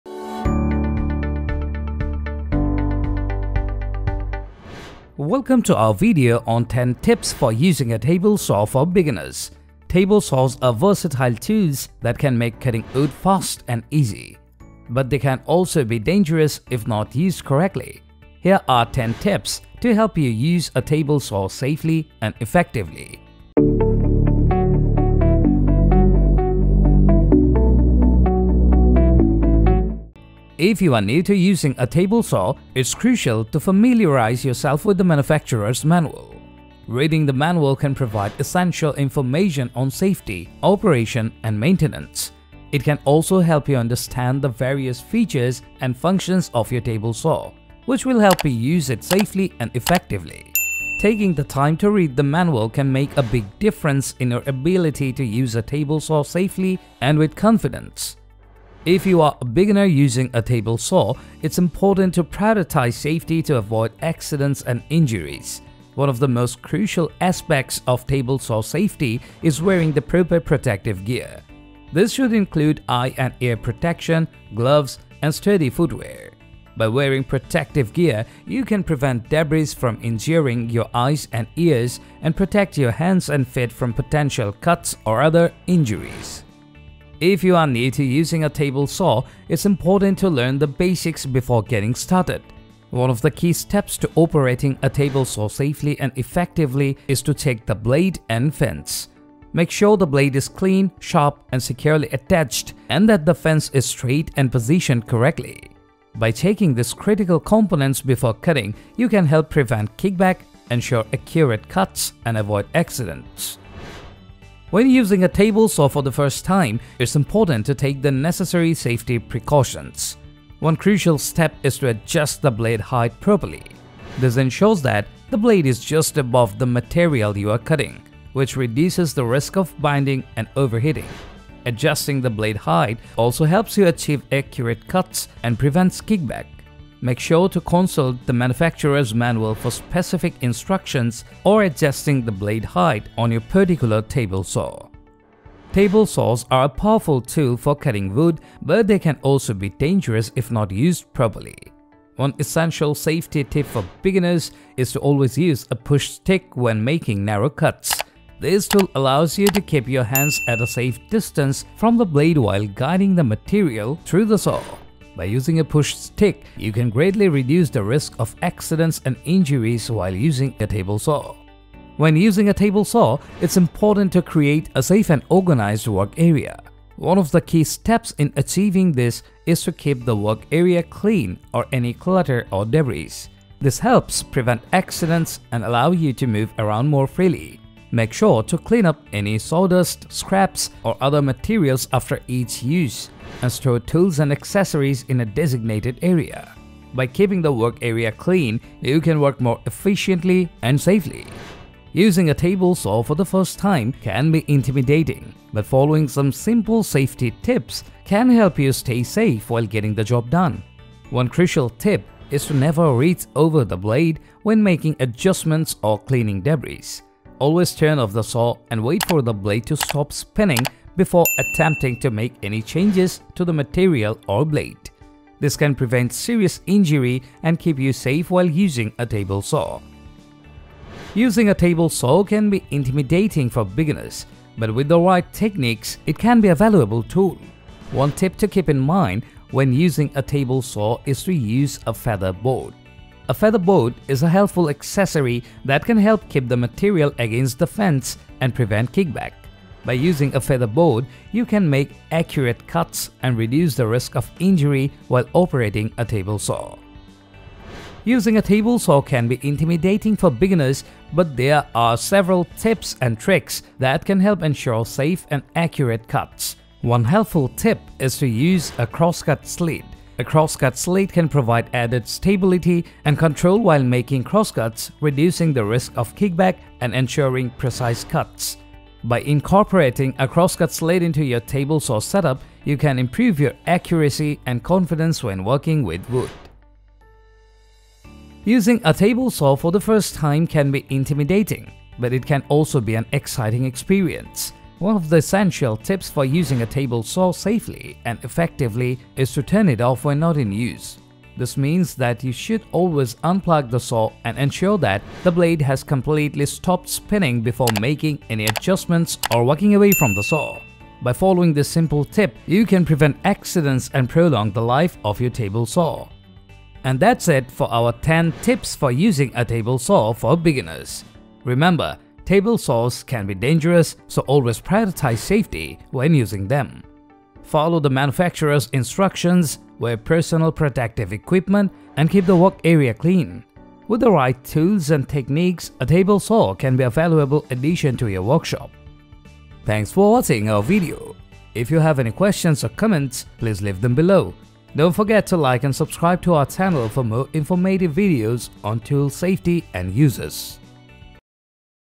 10 Essential Table Saw Tips for Beginners | Master Your Woodworking Skills!
Are you new to woodworking and looking to make the most of your table saw? You've come to the right place! In today's video, we're sharing 10 essential tips to help you use a table saw safely and efficiently. 🛠️🌳

Follow along as we guide you through the basics of using a table saw, ensuring you have the right safety gear, understanding different cuts, and selecting the proper blades. We'll also cover essential techniques, tips for preventing kickback, and how to maintain your table saw for long-lasting performance.

Have questions or suggestions? Drop them in the comments below, and we'll be happy to help!